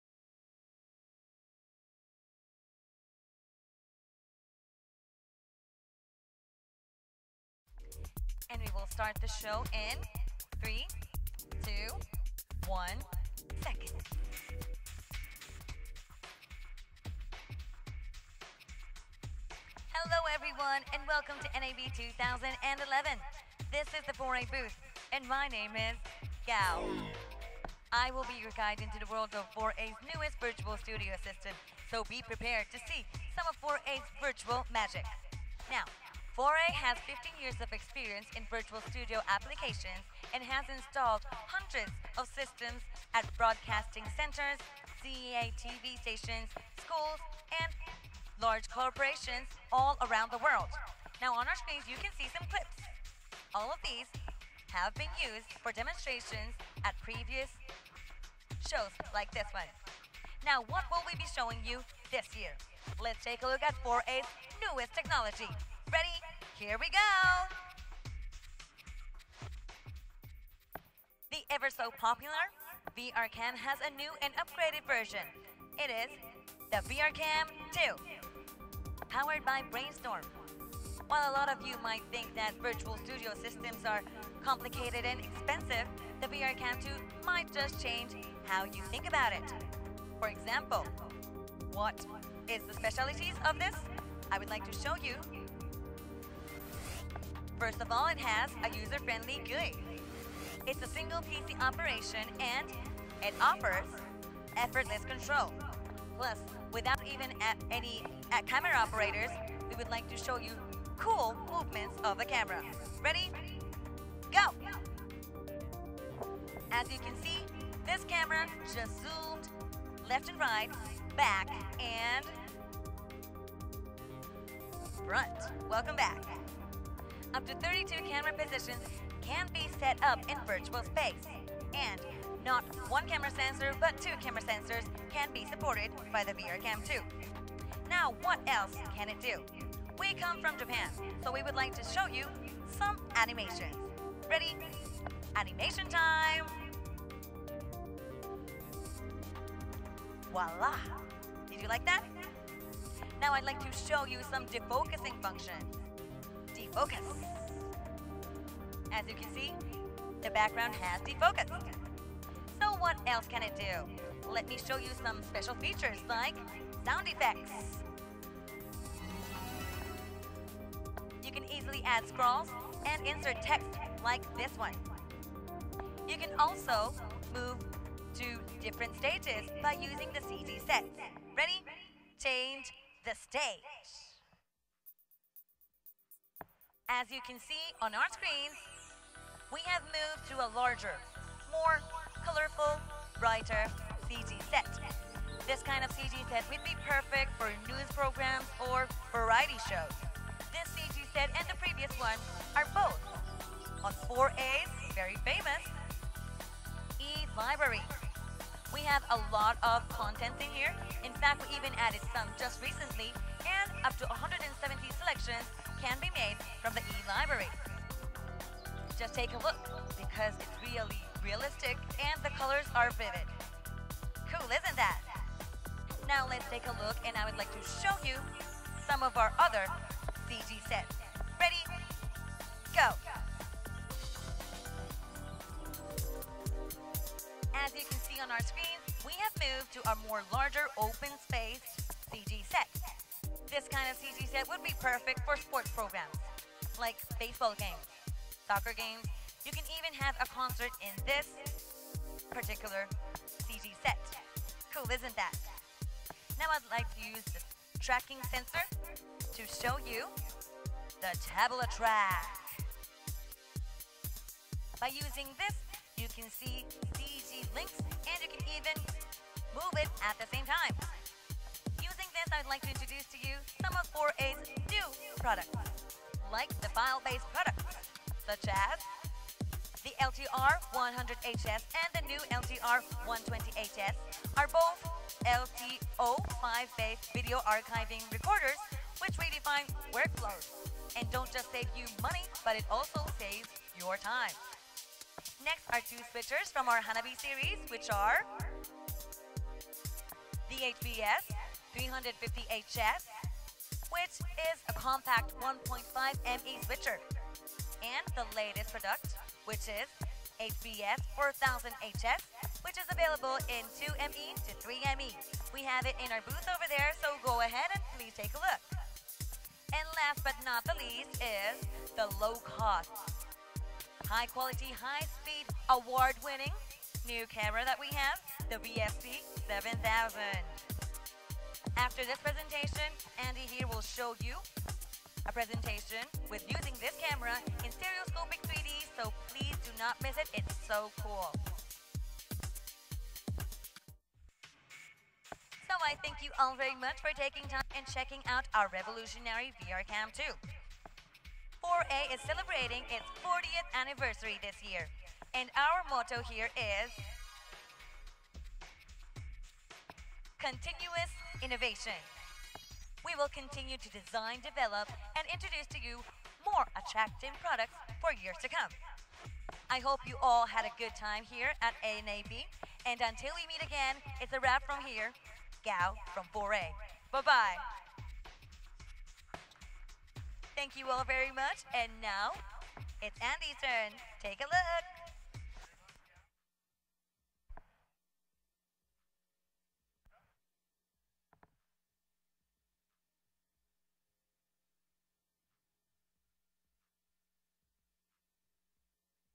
FOR-A VRCAM2 Sensor-less Virtual Studio System DEMO NAB 2011
VRCAM, FOR-A's sensor-less virtual studio system enabling a virtual studio using a one-man-operated, no-cameraman environment, has been updated to the VRCAM2 with even more advanced features. The VRCAM2 system includes expanded camera positions, sensored cameras, and a host of other useful additional functions for program production.

Basic configuration
- PC: Z800 (Hewlett-Packard) or equivalent product
- Graphics card: FX5800 (NVIDIA) or equivalent product
- CG processor: MBP-1244 (FOR-A), Chroma keyer: MBP-12CK (FOR-A)
- VRCAM2 software (VRCAM2 Proc, VRCAM2 Cont)
- Brainstorm eStudio license (Selectable from Engine Save, Desktop, and Global)